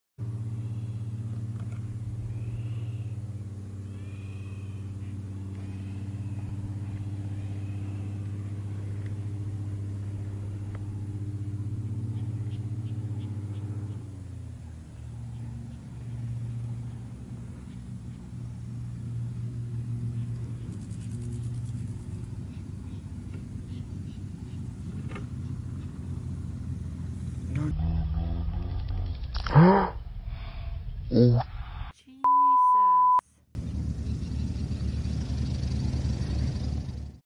Deer Runs In front of Airplane on Landing
Oh Deer! A deer runs right in front of an airplane on landing.